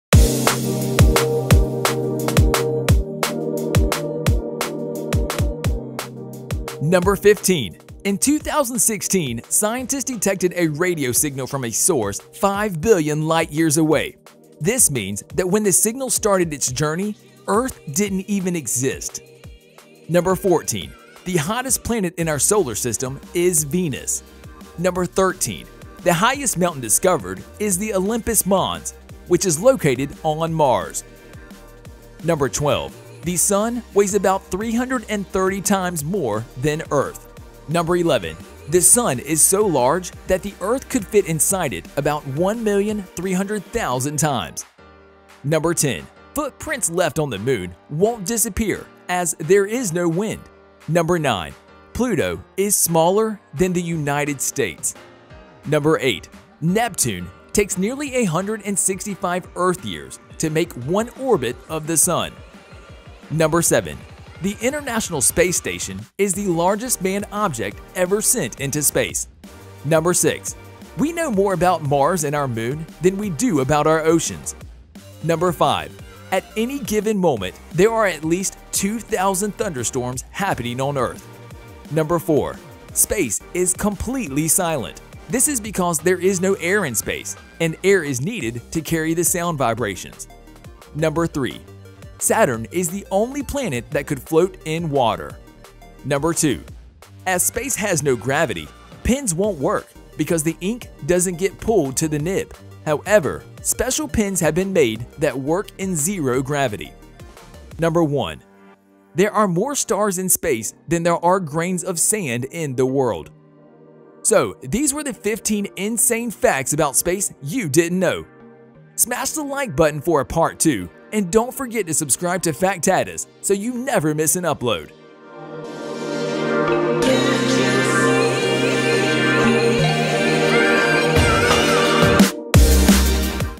15 INSANE SPACE Facts You Didn't Know About
Did you know, that the Sun weighs about 330,000 times more than Earth? Well now you know, welcome to the TOP 15 Insane Sprace facts you didn't know.